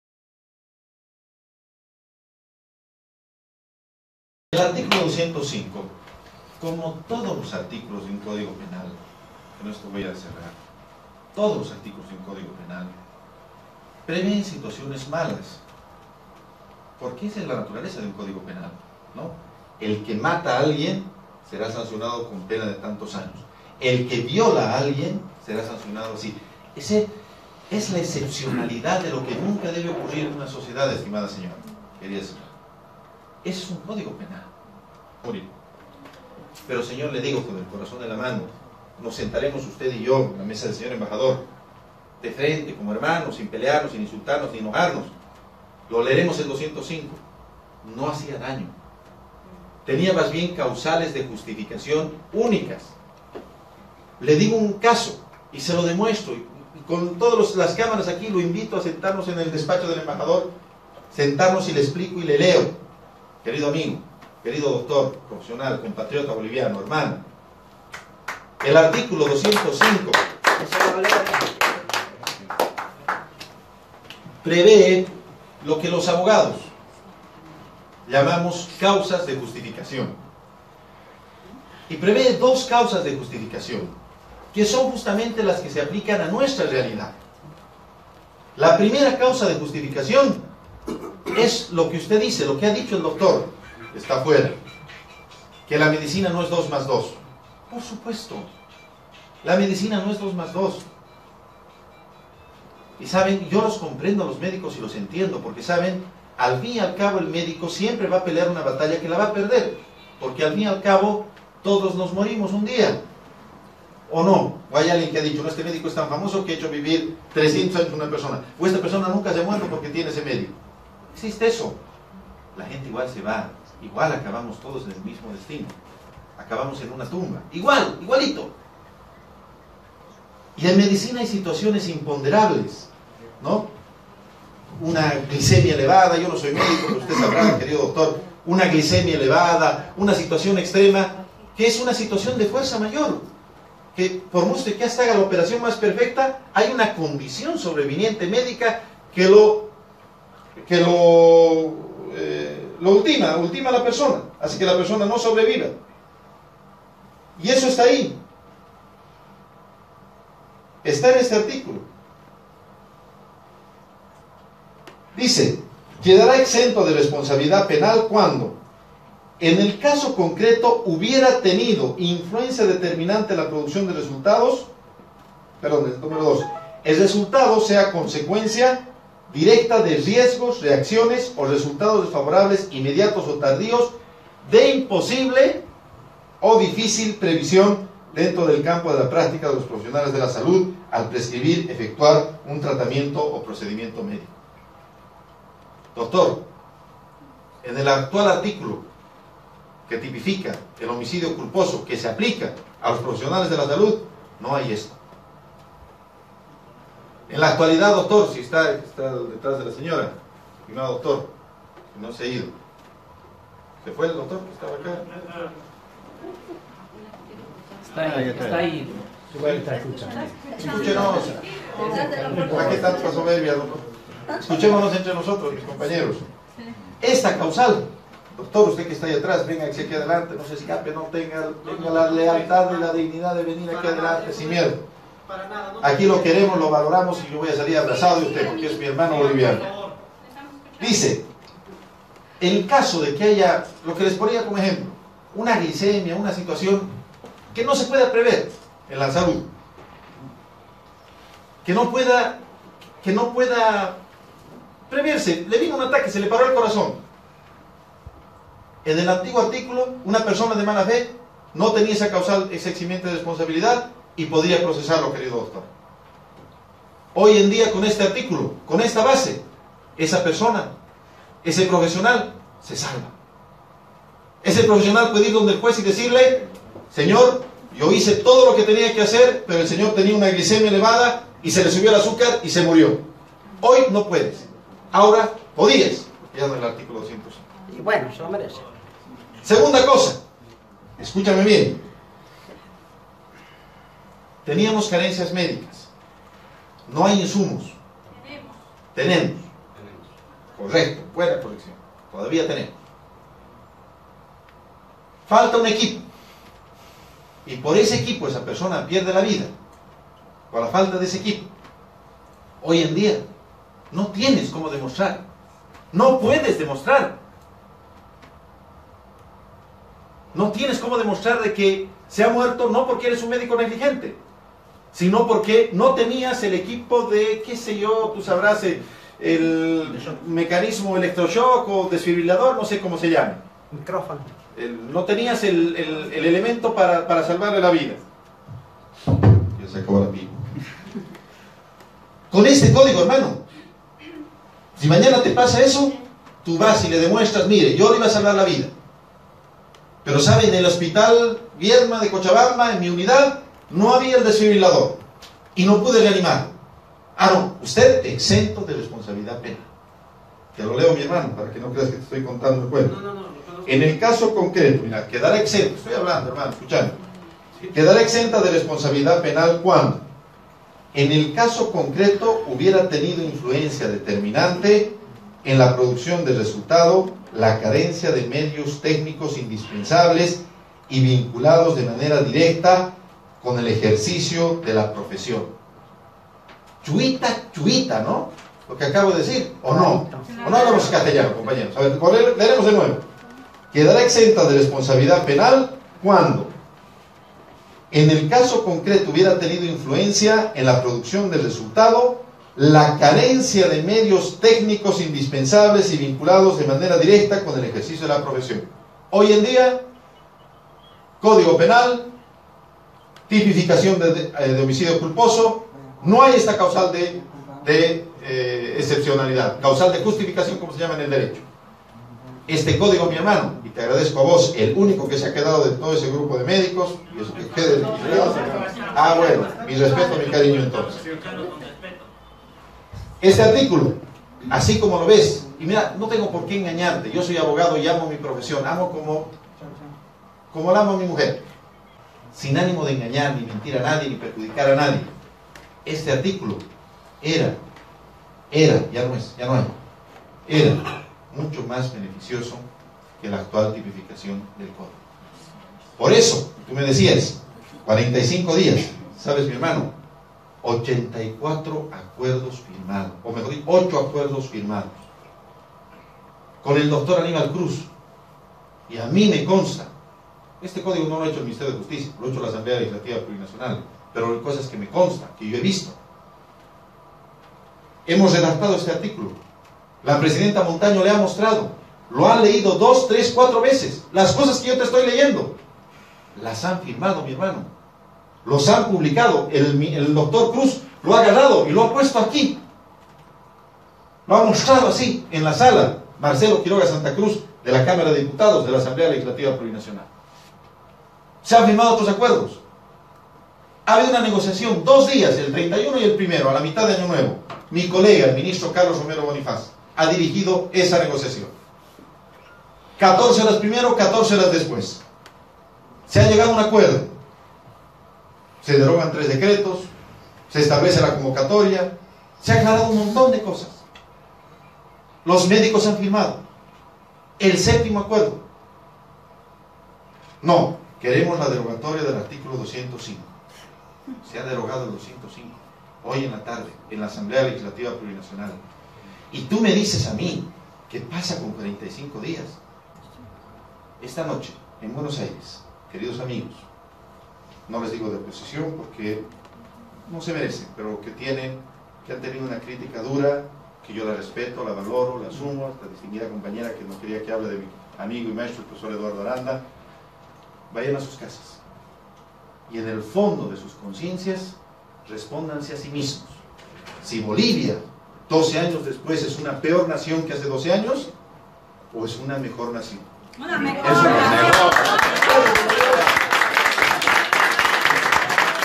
MIN JUSTICIA DESTROZA A MEDICOS
MIN. DE JUSTICIA EN BUENOS AIRES ARGENTINA EXPONE SOBRE LOS ARTÍCULOS DEL CÓDIGO PENAL Y PONE EN EVIDENCIA LA TOTAL IGNORANCIA DE LOS MÉDICOS.